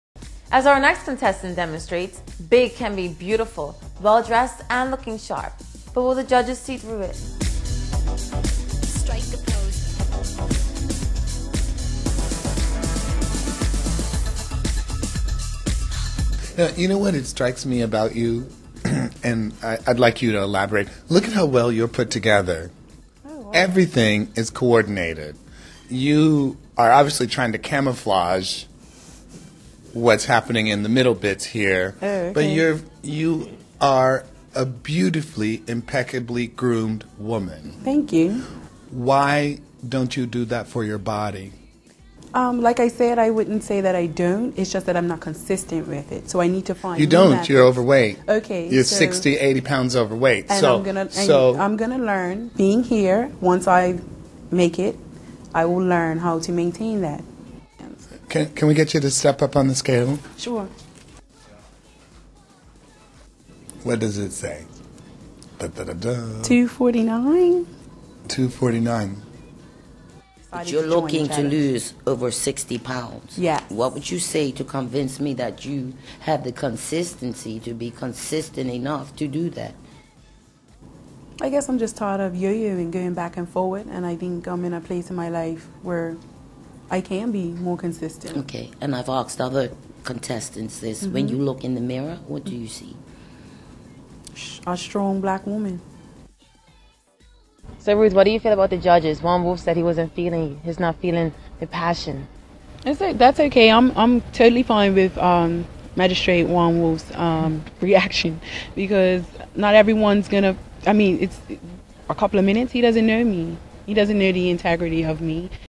Ruth Anne Hunt
The BFM Bermuda 100 Day challenge interview with Ruth Anne Hunt